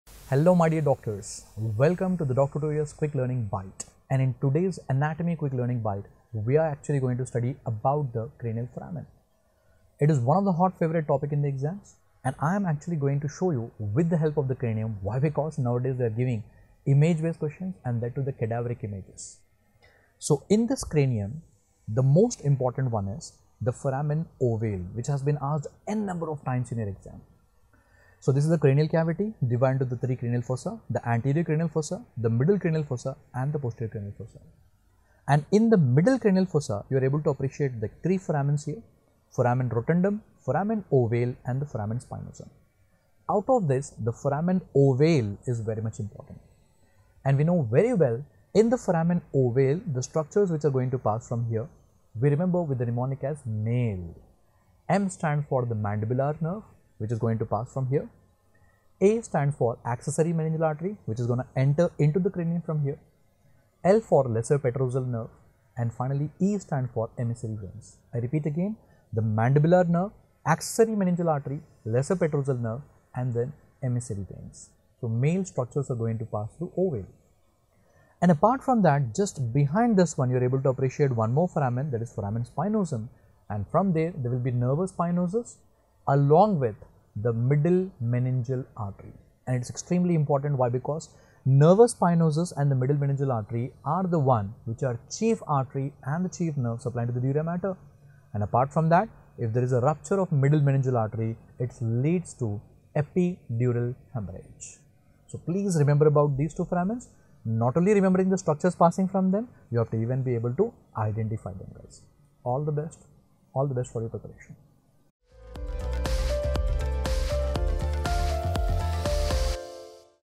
Anatomy- Cranial Foramen | Dr. Mohammed Azam | Quick Learning Bites | NEET PG | FMGE | DocTutorials
Watch Quick Learning Bites to understand Cranial Foramen from Anatomy explained by Dr. Mohammed Azam.